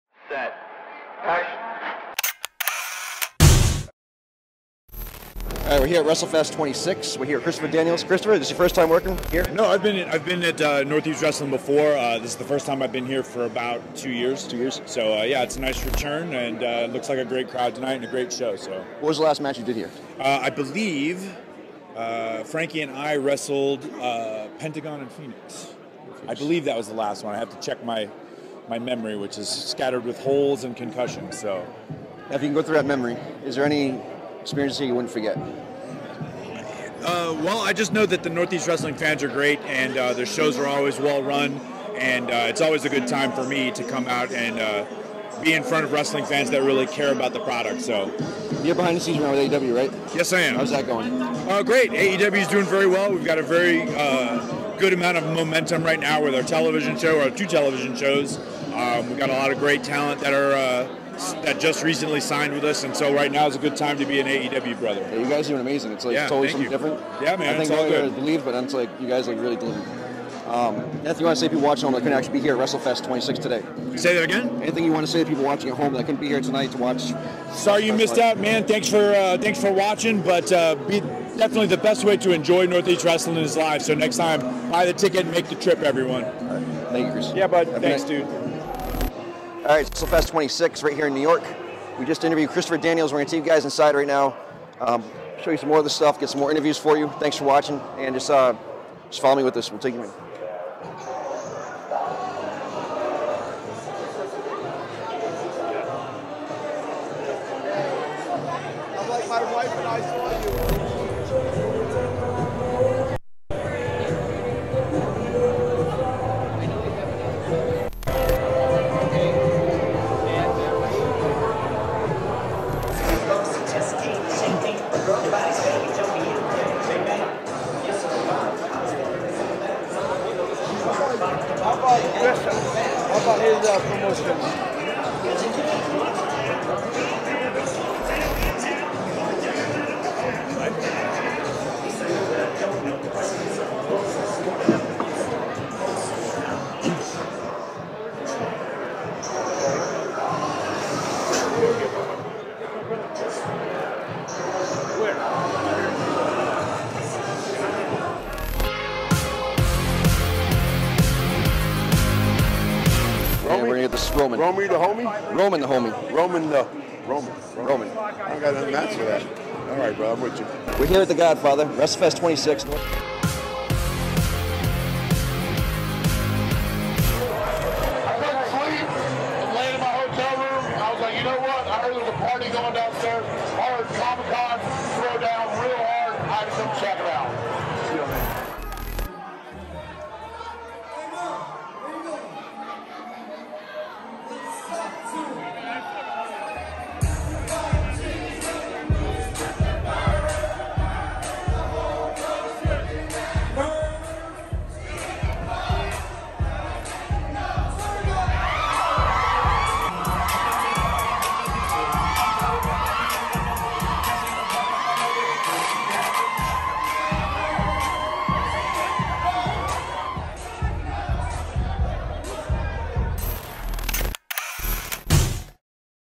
AEW Christopher Daniels Interview Northeast Wrestling Wrestlefest 26 2022 Vangeli Media
Westlefest26 interview with AEW Superstar Christopher Daniels,. Talks AEW experience and past Northeast Wrestling matches, memories, working with the Lucho Brothers, and more.